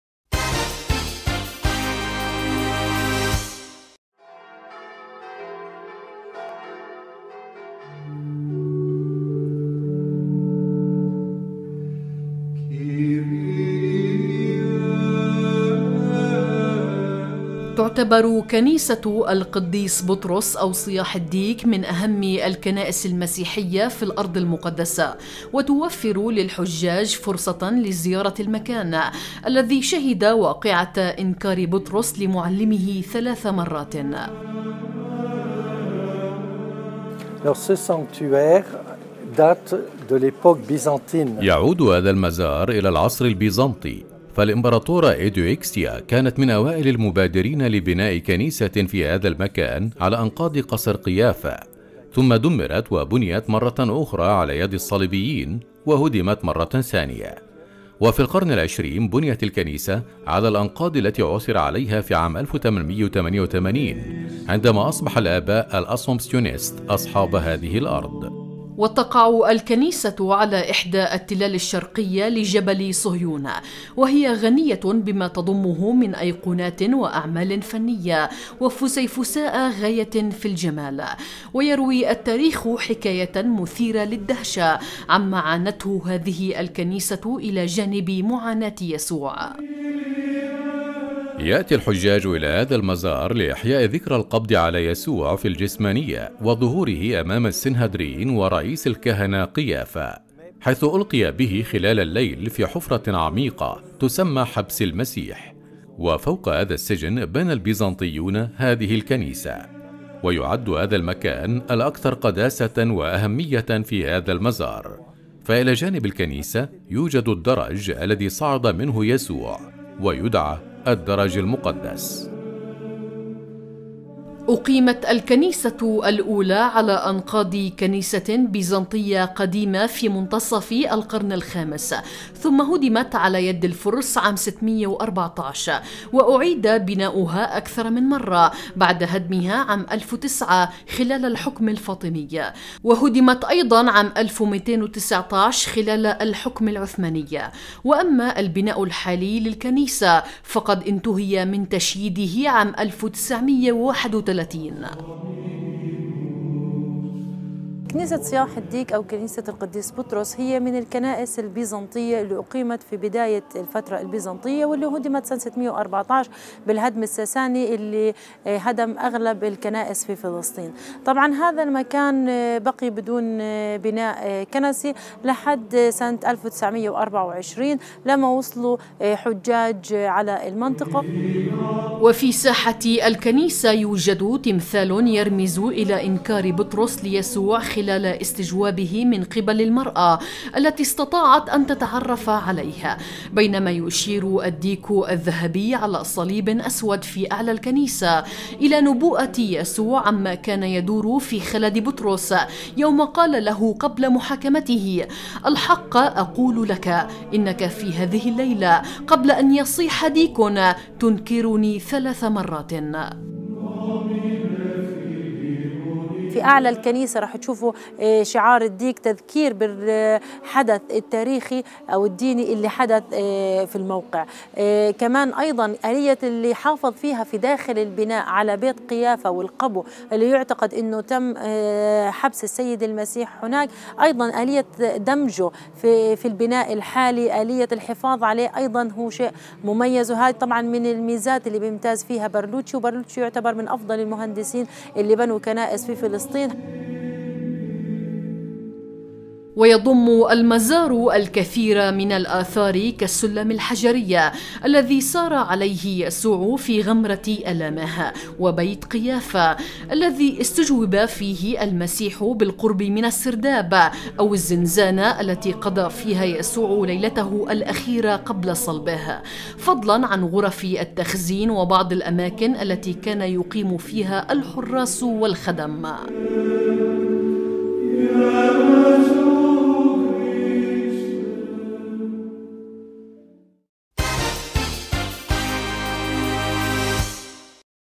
كنيسة صياح الديك وإنكار بطرس ليسوع
من بين المزارات الأكثر أهمية في موطن يسوع مزار صياح الديك الذي شُيّد إحياء لذكرى إنكار بطرس ليسوع ثلاث مرات كما يروي الكتاب المقدس.